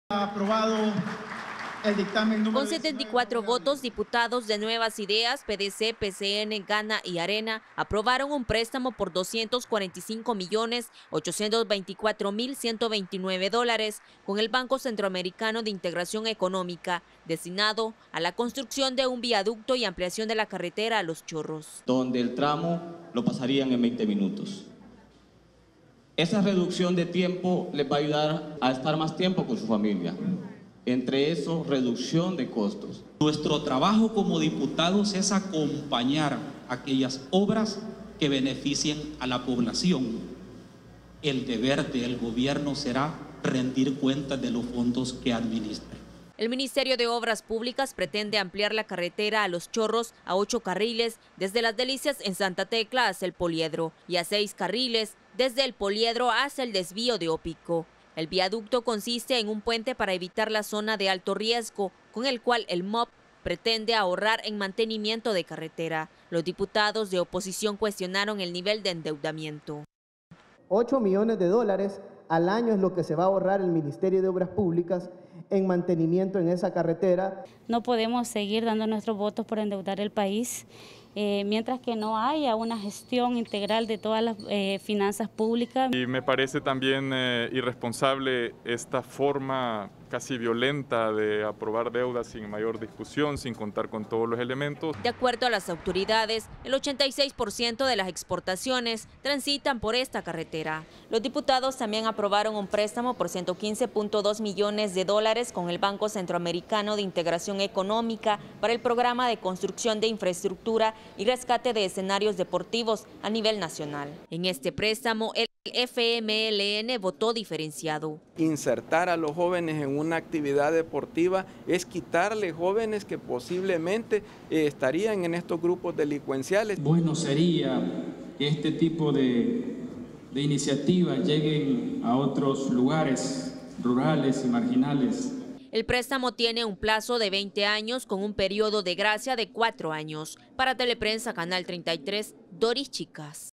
Teleprensa33 | Aprueban más de $245 millones para ampliar carretera Los Chorros
La Asamblea Legislativa aprobó adquirir más deuda y esta vez por 361 millones de dólares para deportes y ampliación de la autopista Los Chorros.